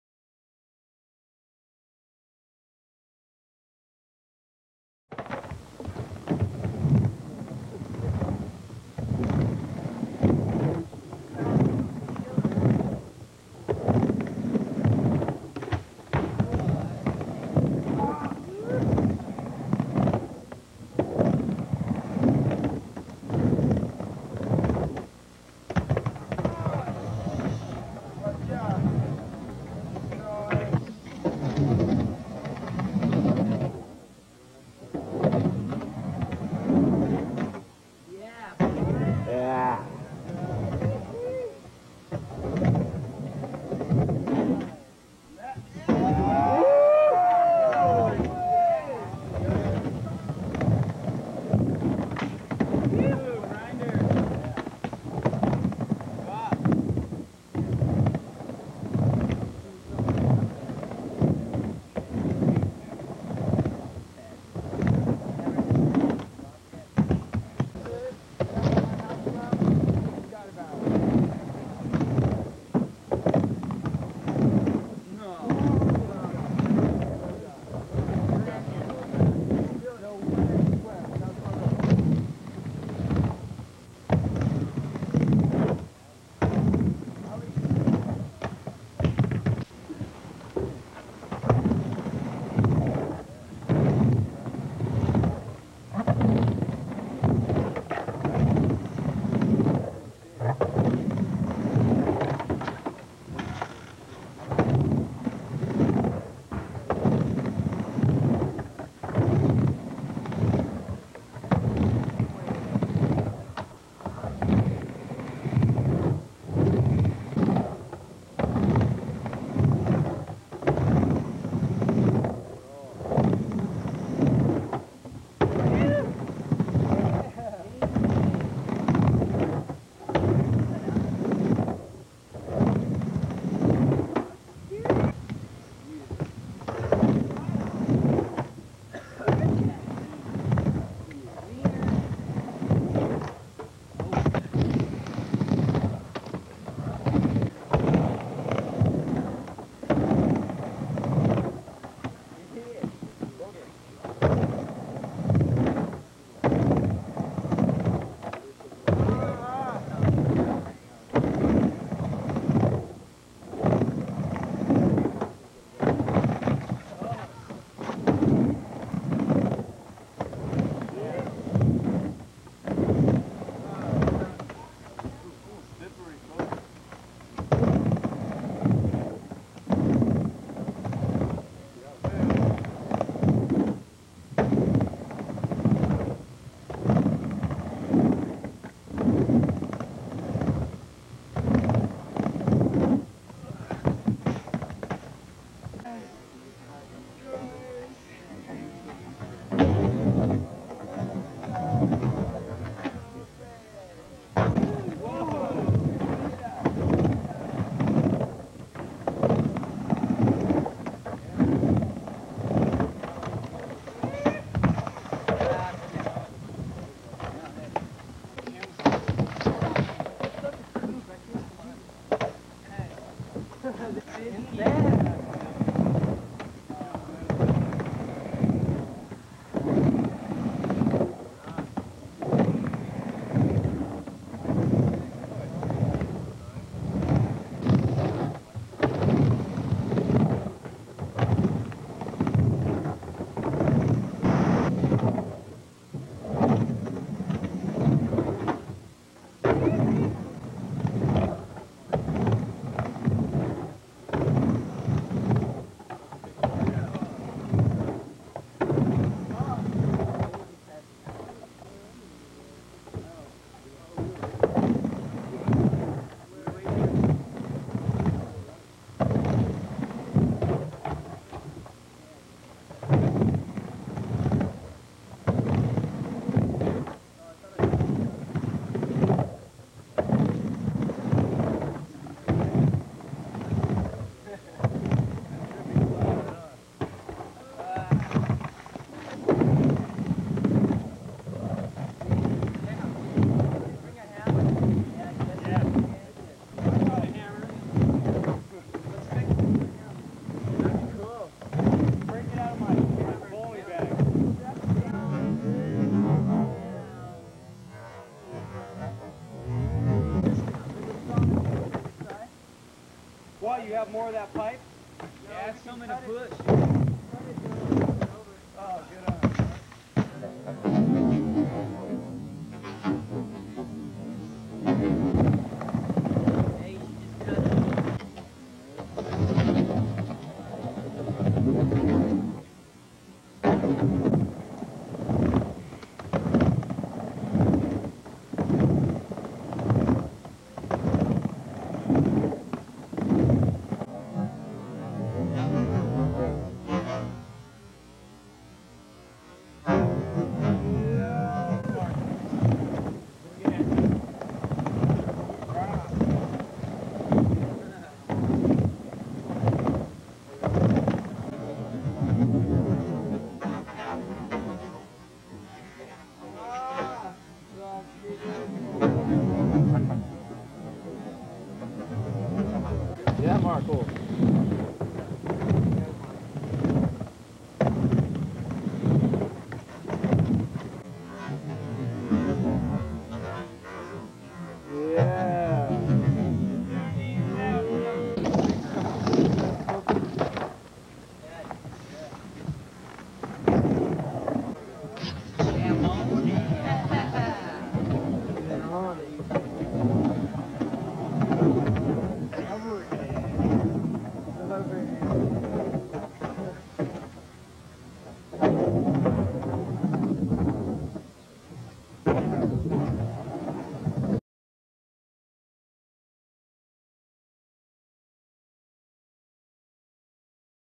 Classic Sessions - Barranca Ramp
Old school skate session at Ventura's barranca Ramp from the early 80s with Jesse Martinez, Doug DeMontmorency, Marc Hollander, and some other local shredders.